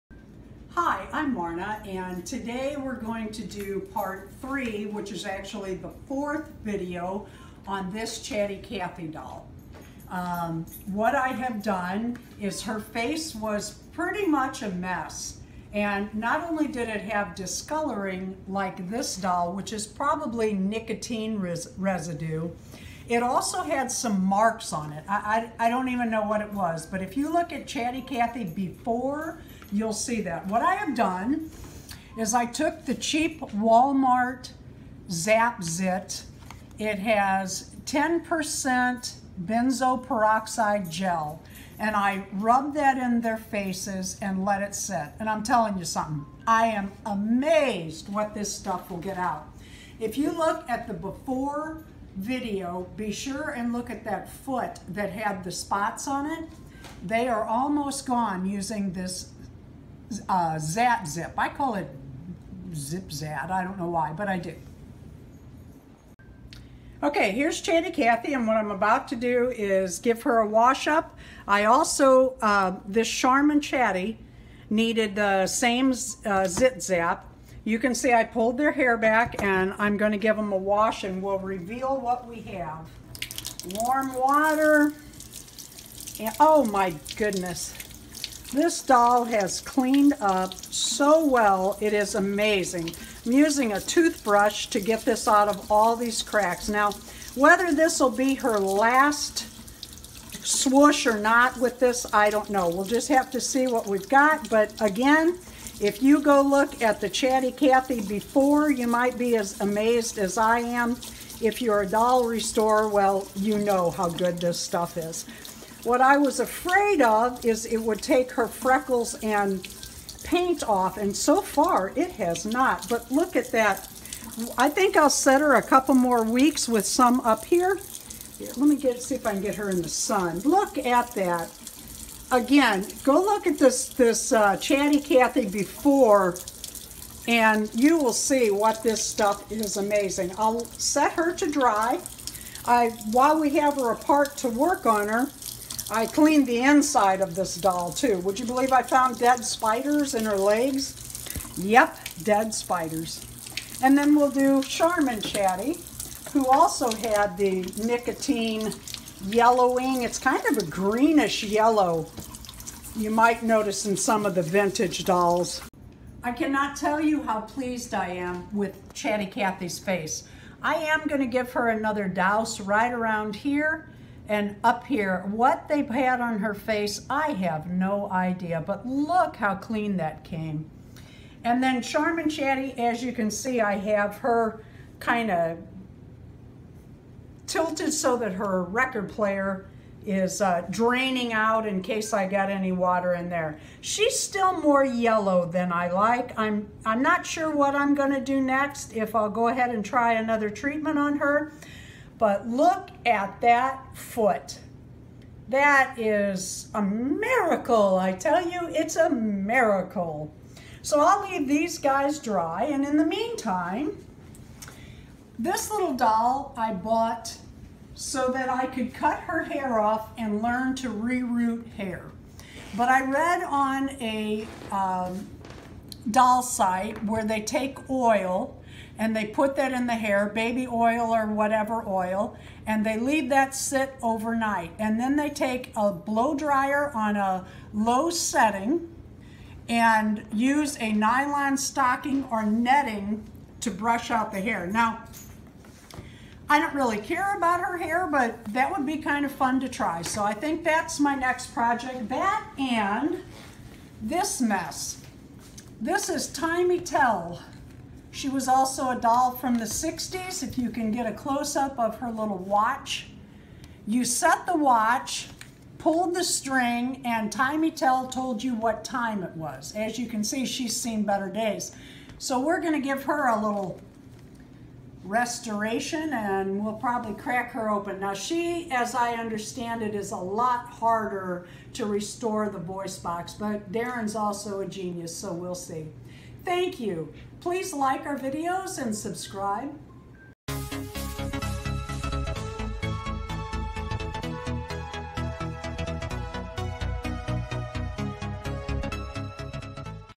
Chatty Cathy Restoration Part 3 and Charmin Chatty
Part 3 (4 counting the Before Video) of our first Chatty Cathy Restoration.  Little by little she gets cleaned up and fixed up.

Watch the previous videos in this series!